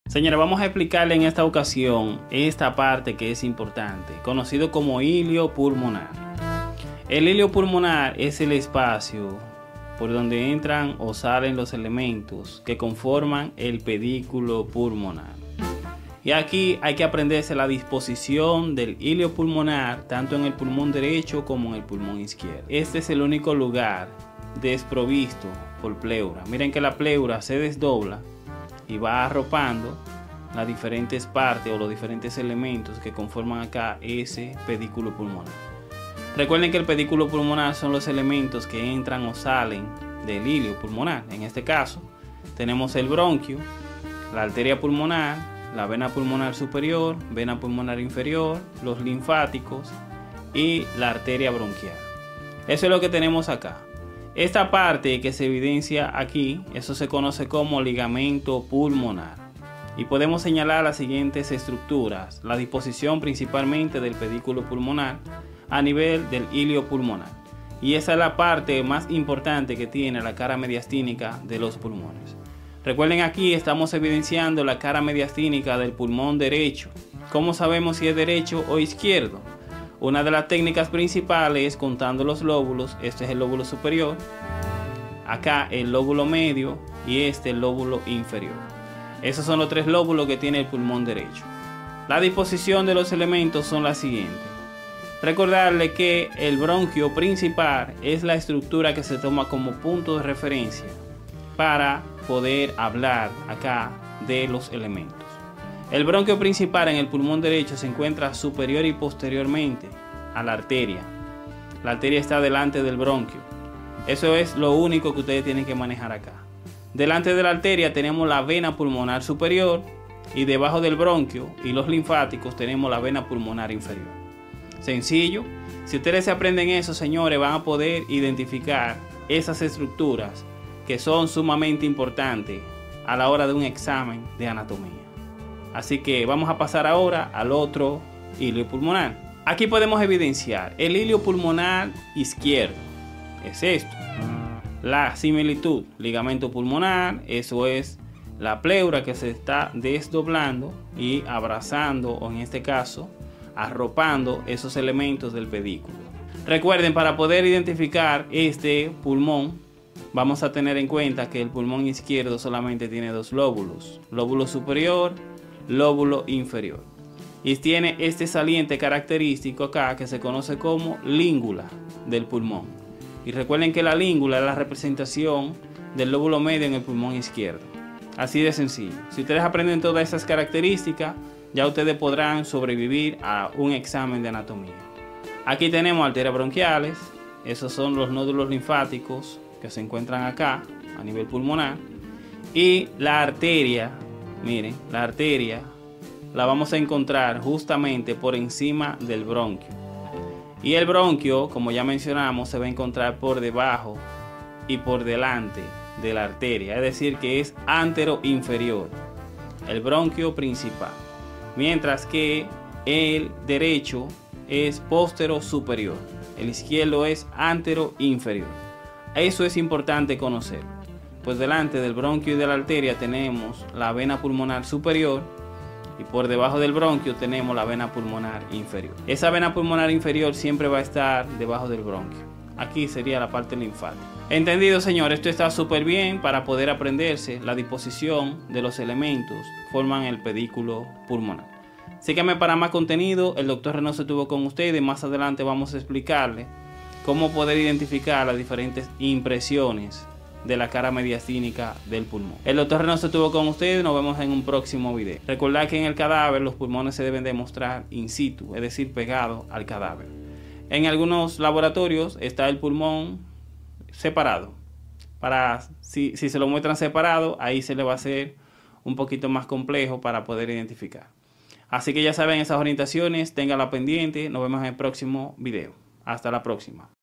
DISPOSICIÒN DE LOS PEDÌCULOS PULMONARES, HILIO PULMONAR
Hola en este vídeo te explico la importancia de como aprenderte la disposición de los elementos del pedículo pulmonar derecho e izquierdo.